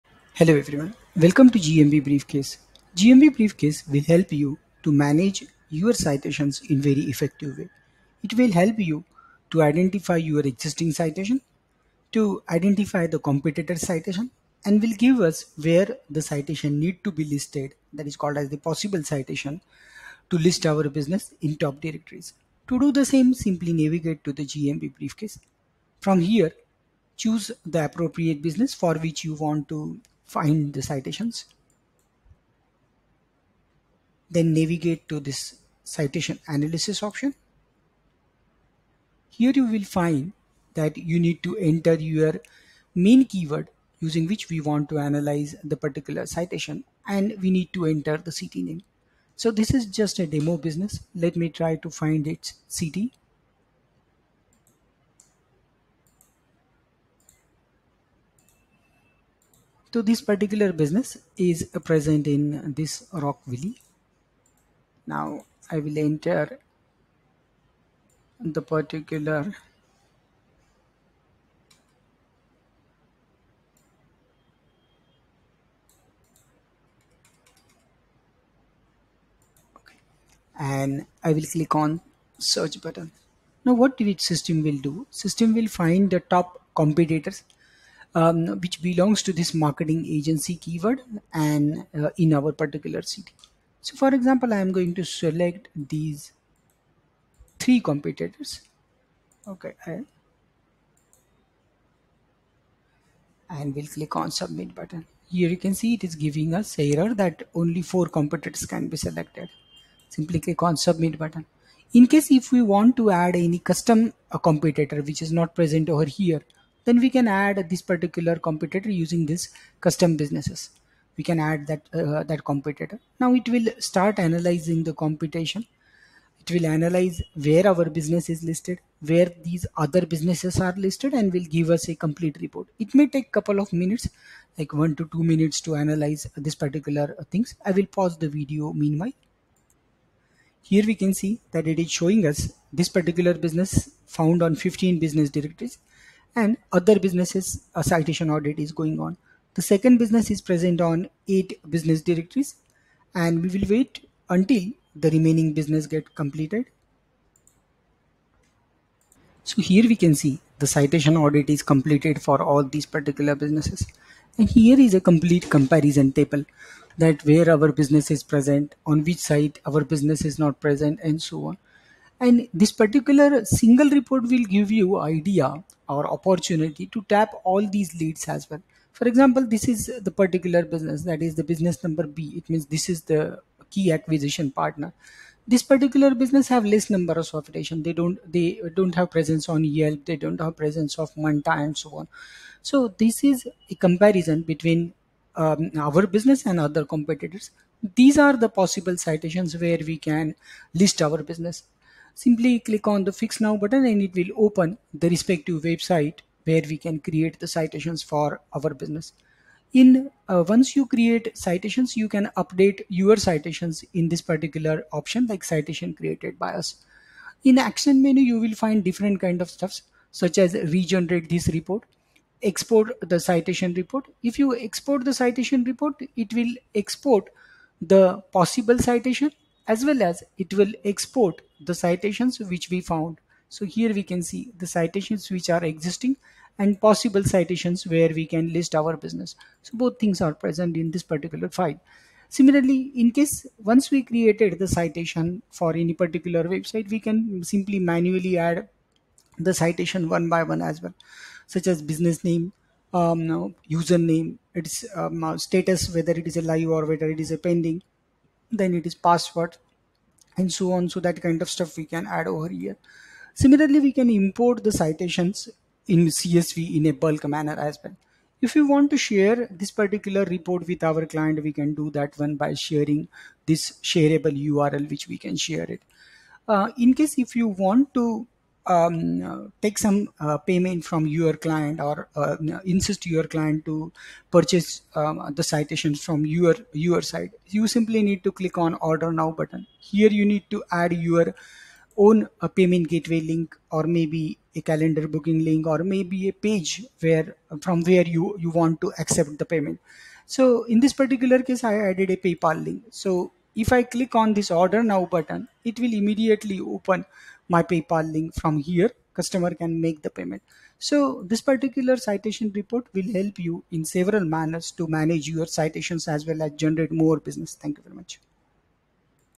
A detailed overview of the citation module?
In this video, dive into a detailed overview of the citation module! Discover how to manage and optimize your business citations effectively.

Click now to elevate your local search presence and attract more customers!